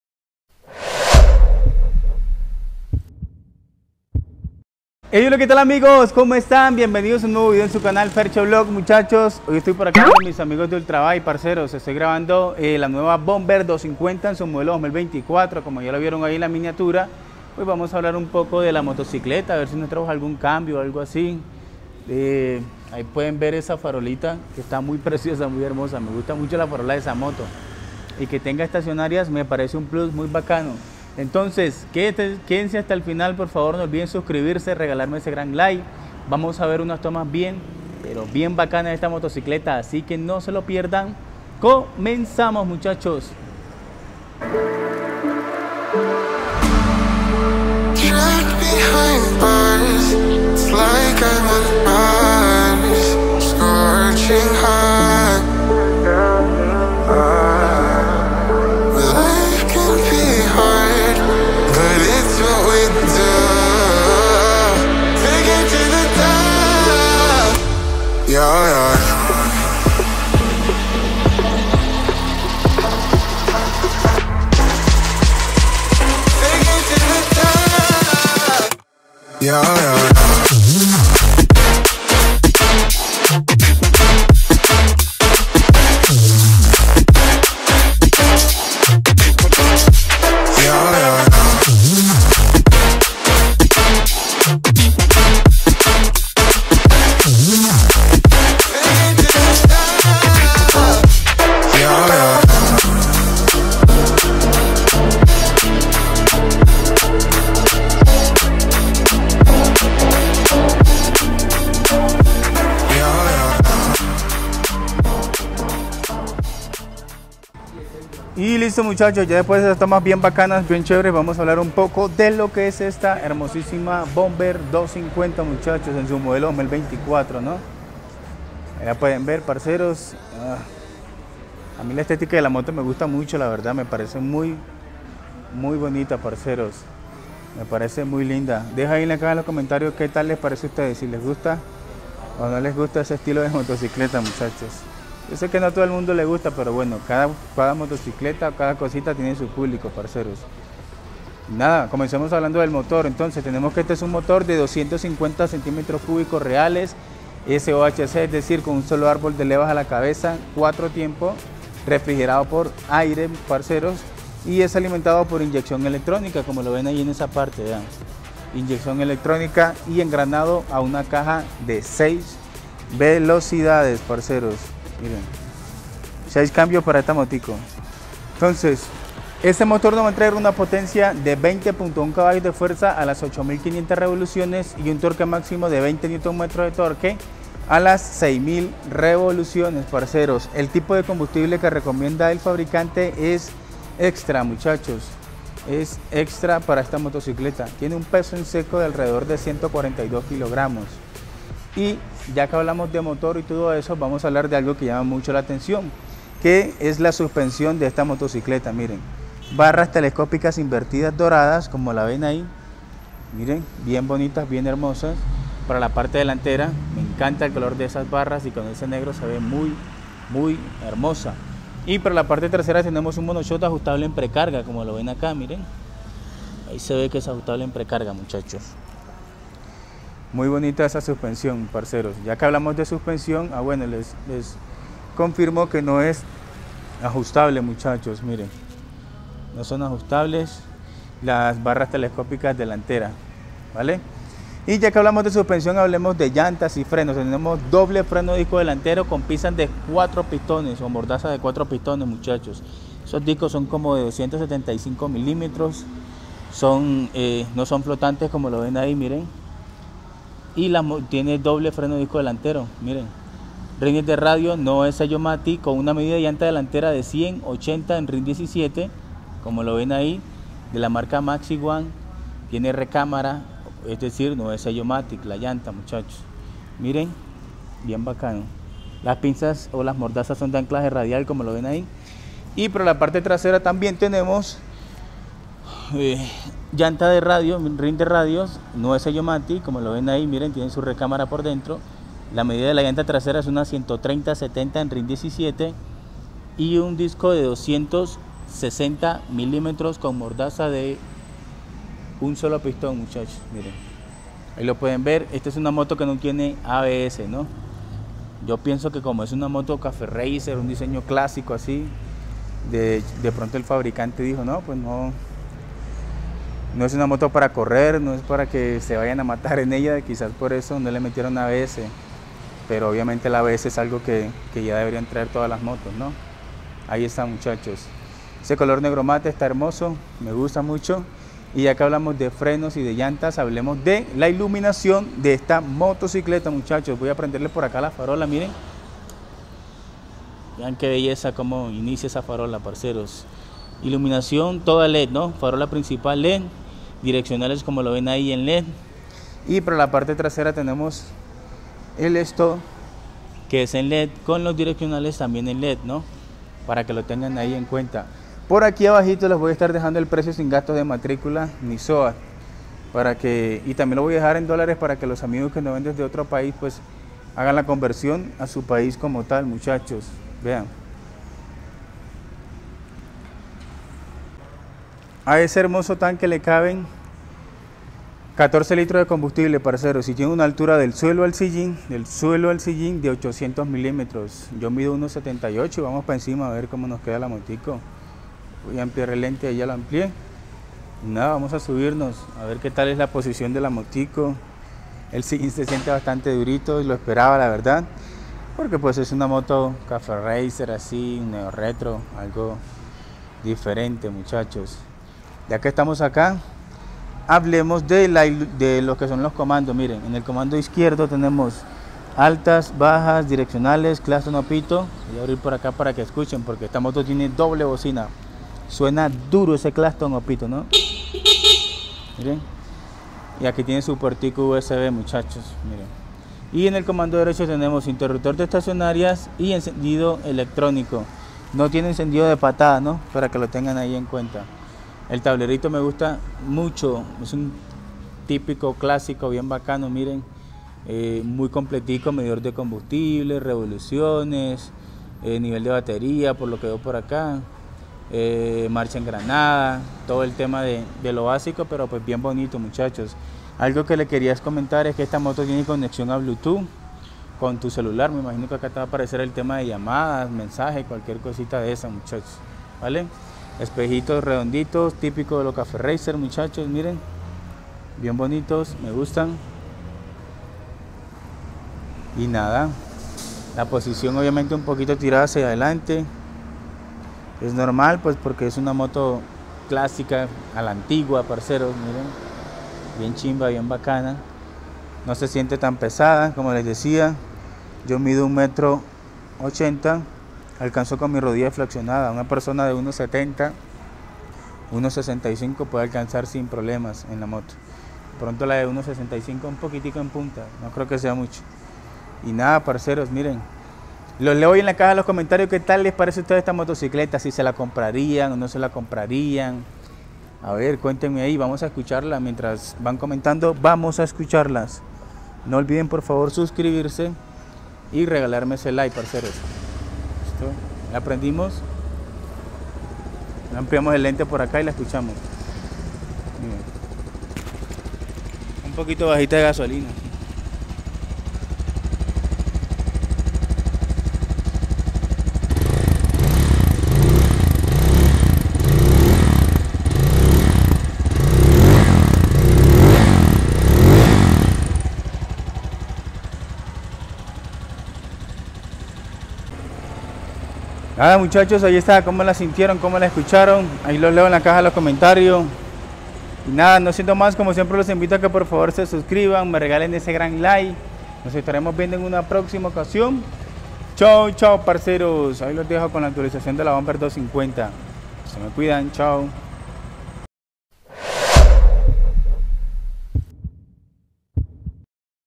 Victory Bomber 250 modelo 2024 el tanque de auteco en mediano cilíndraje 🔥 les gusta esta moto ? 🤔
Auteco es una ensambladora colombiana que hace más de 80 años está distribuyendo motos en el país y se lanzó con su marca Victory y esta nueva Línea que llama mucho la atención como marca propia